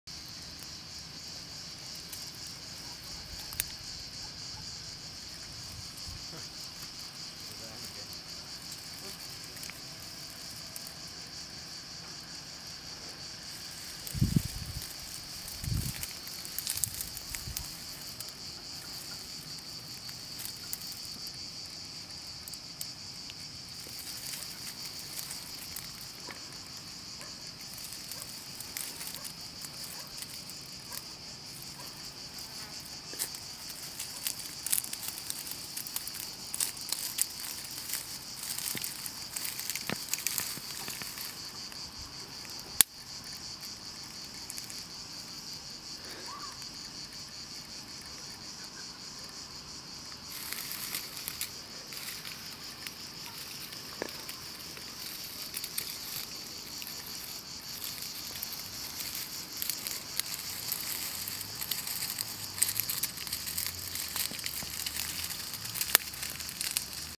Testudo hermanni in Ammouliani
A Hermann's tortoise on the island of Ammouliani.